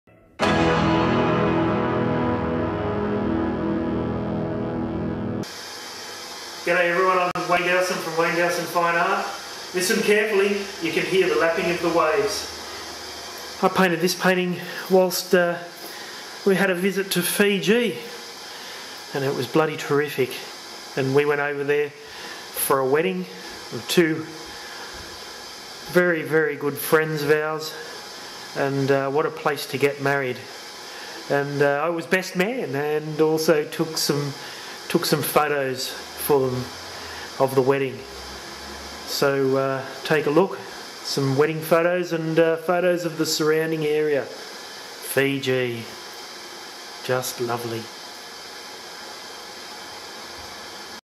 Fiji Painting.
Wayne visited wonderful Fiji...and was inspired! Fijī is an island nation in Melanesia in the South Pacific Ocean about 1,100 nautical miles (2,000 km; 1,300 mi) northeast of New Zealand's North Island. Its closest neighbours are Vanuatu to the west, France's New Caledonia to the southwest, New Zealand's Kermadec to the southeast, Tonga to the east, the Samoas, France's Wallis and Futuna to the northeast and Tuvalu to the north.